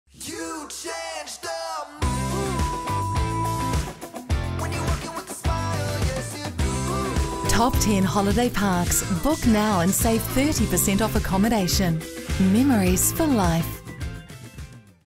TOP 10 Holiday Parks - 72 hour sale - 30% off during Spring 2022
Enjoy 30% off accommodation and sites at TOP 10 Holiday Parks across New Zealand during Spring 2022. TOP 10 Club members only when booked directly